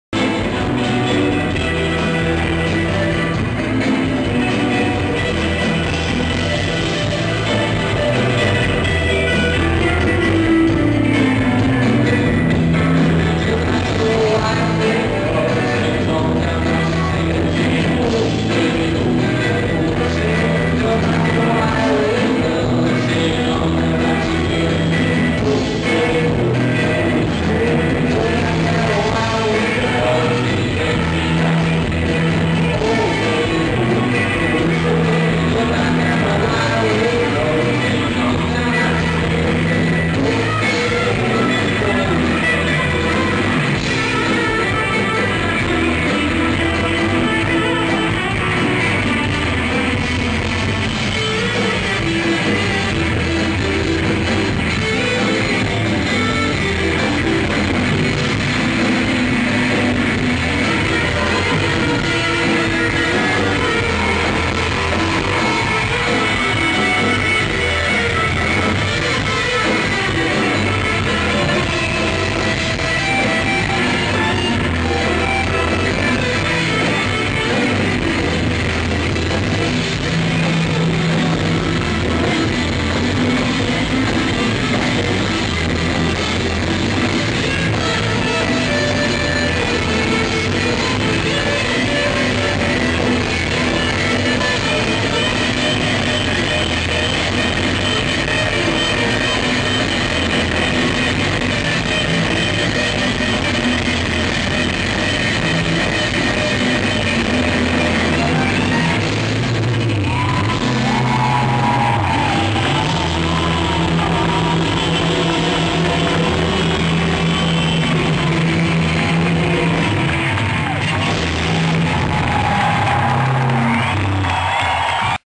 Devendra Banhart - Live @ Festival des Inrocks 2007 Olympia
Devendra Banhart - Live during Festival des Inrocks 2007 at the Olympia in Paris, the November 12, 2007.